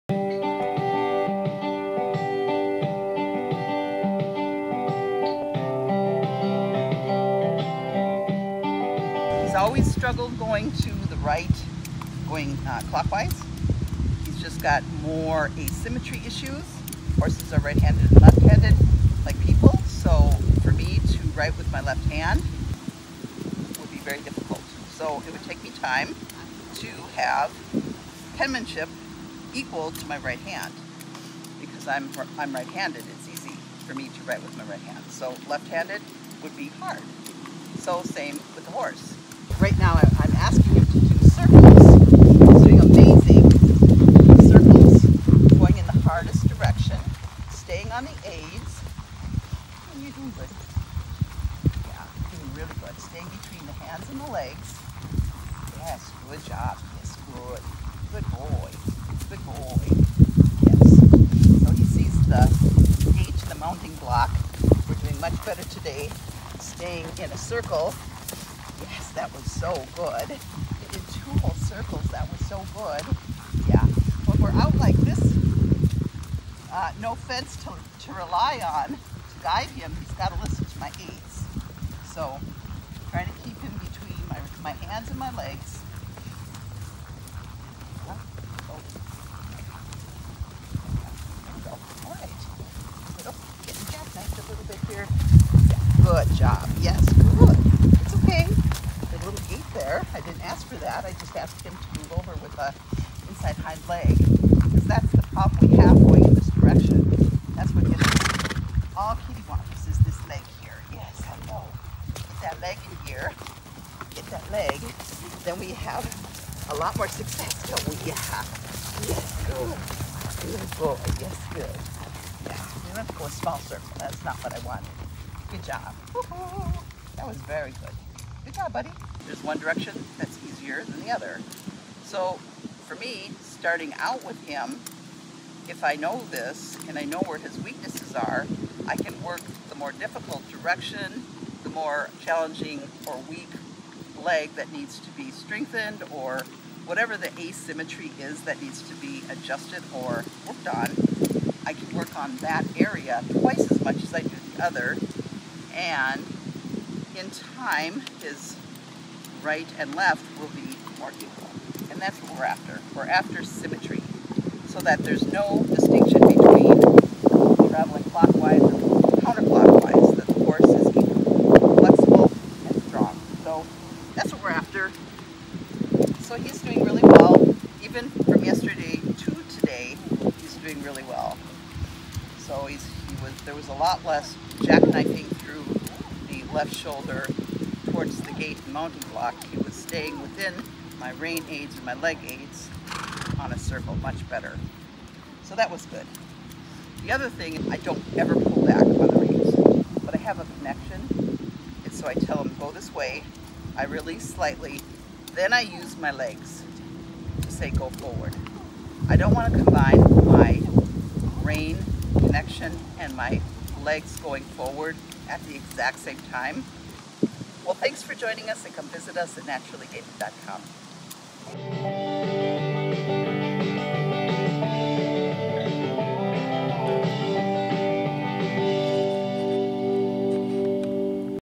Developing Symmetry and the Gaited Horse
In this video, I talk about developing symmetry with the naturally gaited horse. I am riding Marvel, a 4-year-old naturally gaited Tennessee Walking Horse / Spotted Saddle Horse. He has been under saddle for 6 months. I ride him using dressage methods in a snaffle bit, and he is ridden barefoot. The majority of gaited horses and non gaited horses are born right handed or left handed, like people. There are a few lucky ambidextrous horses and people. It isn't me or my horse. So this video talks through working the weaker direction to develop strength and flexibility to help the horse become ambidextrous over time.